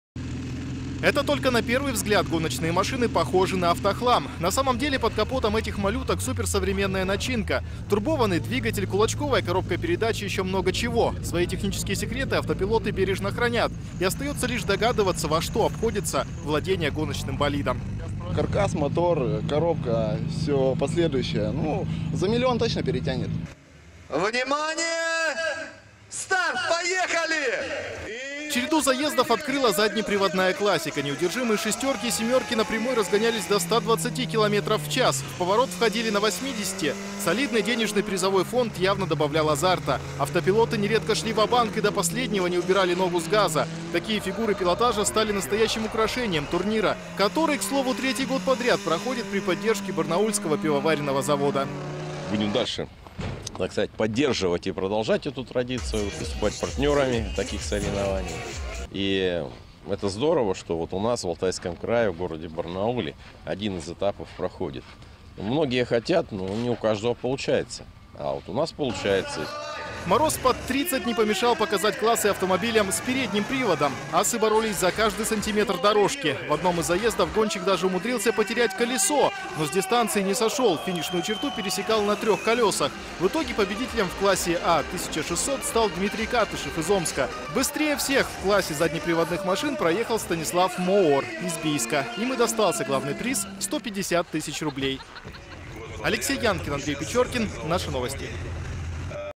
«Кубок Губернатора» по трековым гонкам прошел на барнаульском ипподроме
Смятые крылья, оторванные колеса, аномальные погодные условия. В такой нешуточной борьбе в этот уик-энд мастера зимнего пилотирования из Урала, Сибири и Казахстана выясняли, кто из них достоин называться «лучшим из лучших». Главным призом стал чек на 150 тысяч рублей.